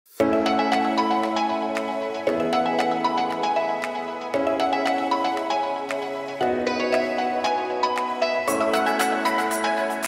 Aesthetic recipe / Coffee Pavlushka with chicory
Coffee Pavlushka with chicory is a combination of coffee and chicory will give you a new look at this drink. Essential ingredients: espresso or coffee, liquid chicory, whipped cream.

       Method:
1️ Brew the espresso/coffee in any preferred way.
2️ Add the chicory.
3️ Top with a cap of whipped cream.